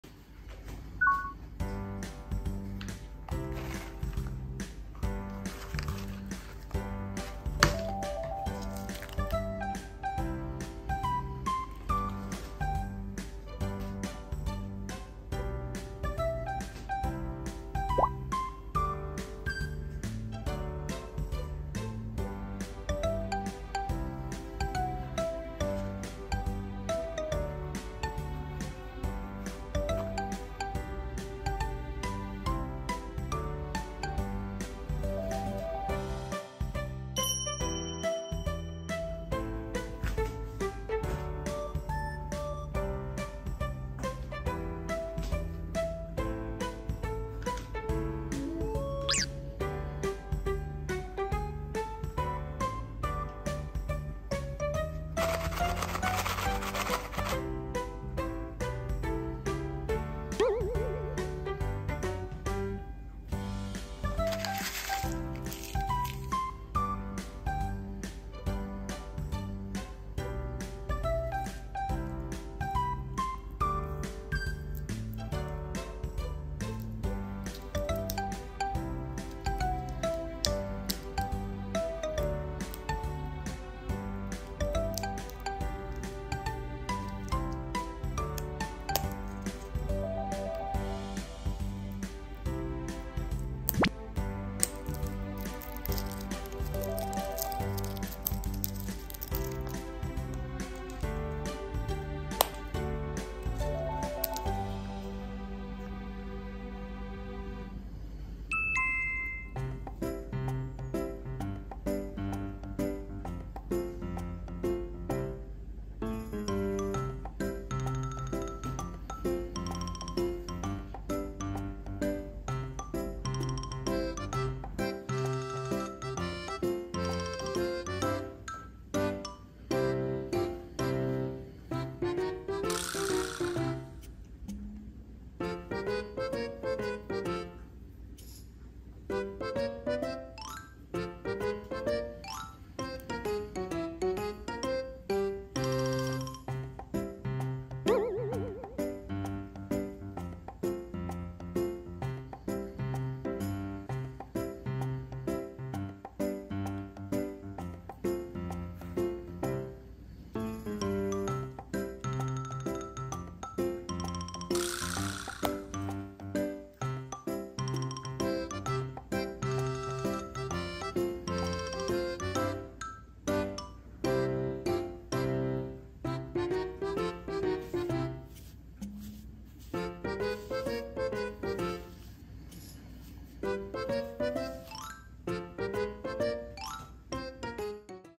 オウムに大豆を見せてみたら困惑しながらくわえてくれて…
こんにちは！ゆきまるちゃんねるです！
ゆきまる氏は基本、大豆を好みません。あんまり食べ物っていう認識をしていない気がします。
ただたま〜にくわえて、崩すだけ。食べることはしません。

BitStarアカデミー タイハクオウムのゆきまる担当者宛